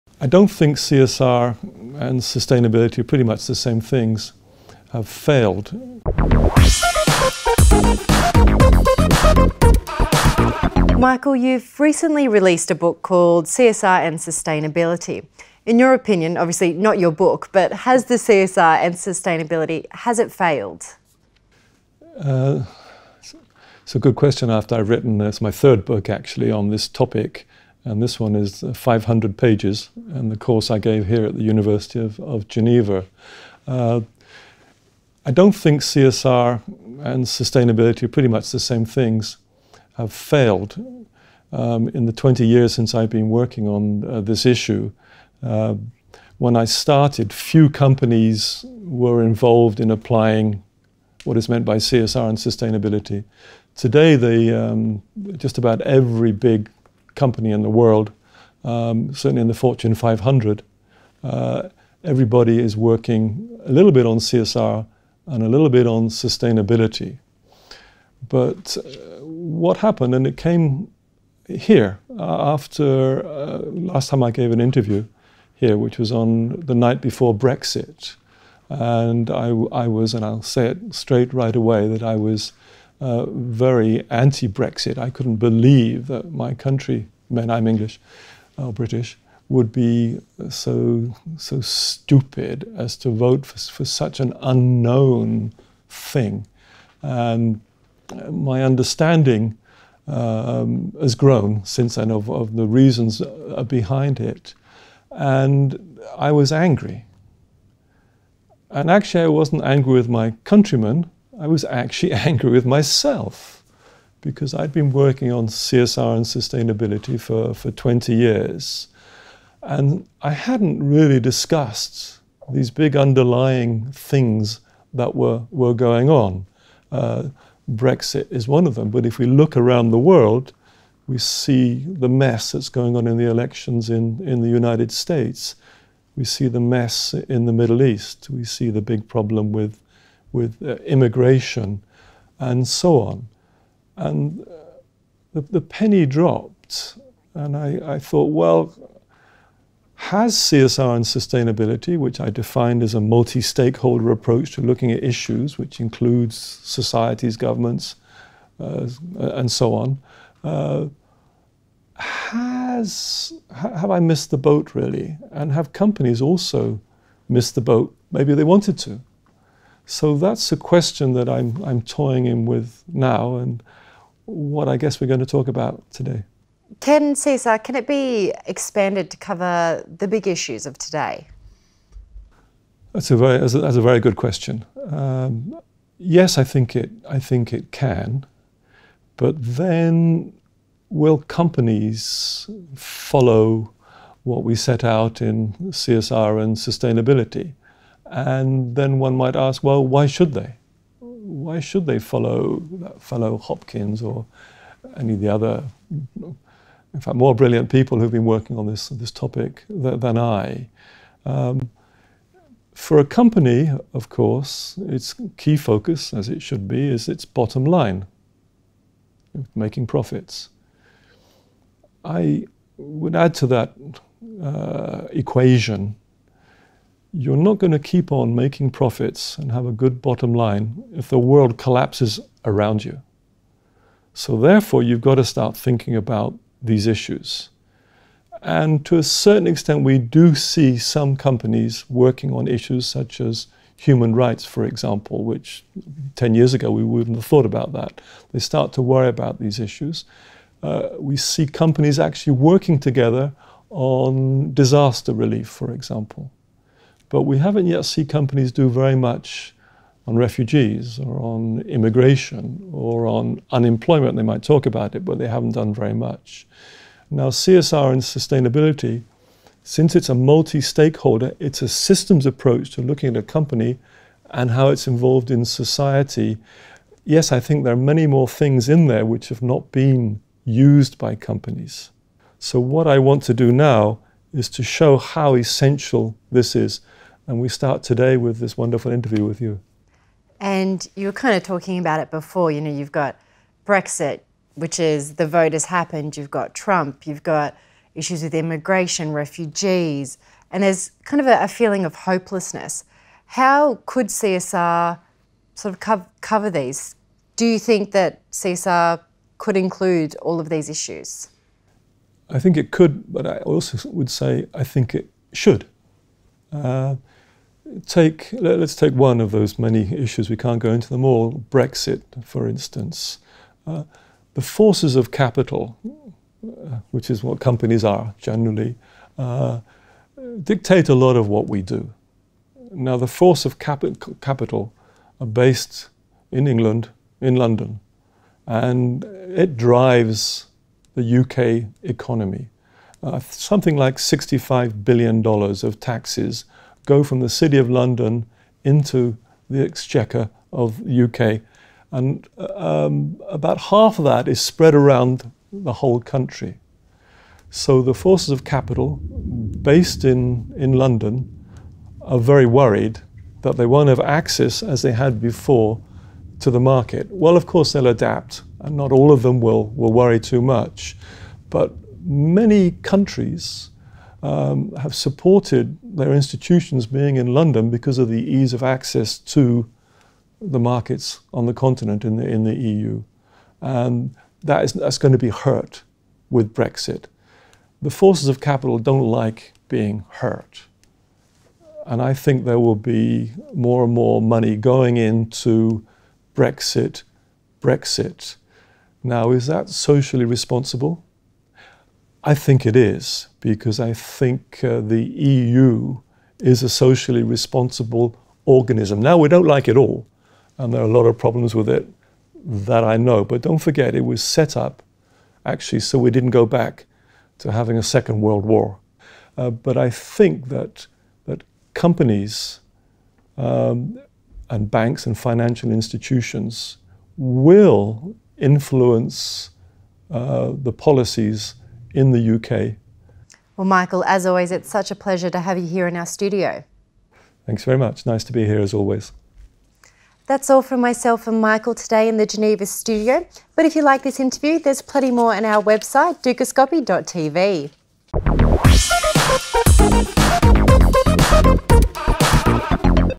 Can CSR address the Big Issues of the Day?
Has corporate social responsibility and sustainability failed? Michael Hopkins, CEO of CSR.